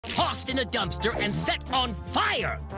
Lord of the Flies should be…
Tossed in the dumpster and set on fire!

And also forced to have the characters from the book be fed Desomorphine.